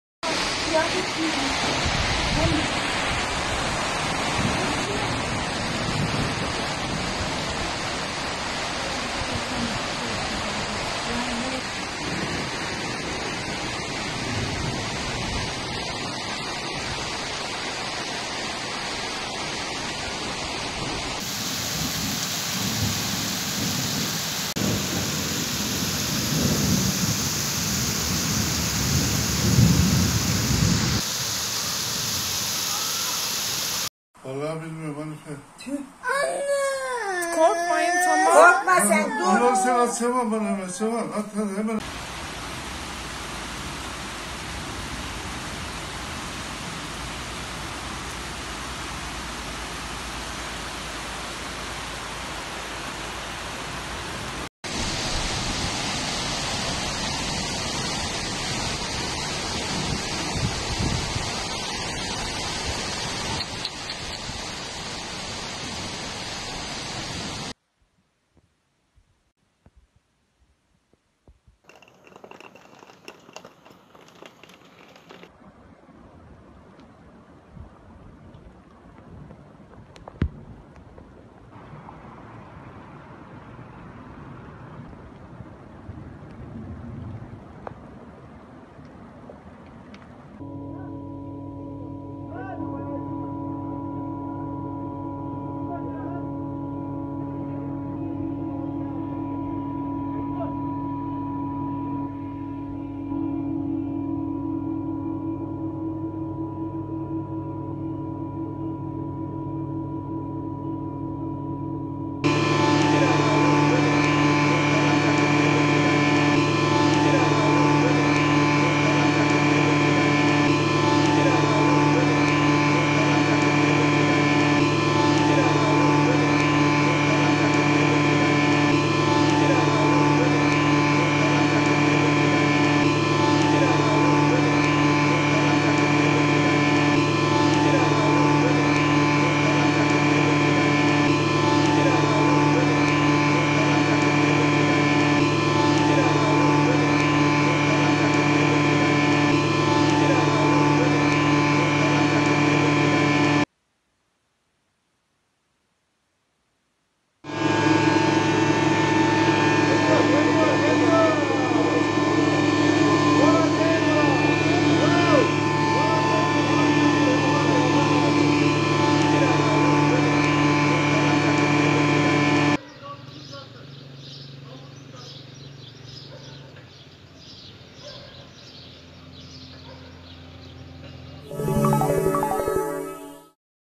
Çankırı'da dün yaşanan sel anı amatör kameralara yansıdı!
Çankırı Tuzlubağlar TOKİ konutlarında dün yaşanan sel anı amatör kameralara yansıdı!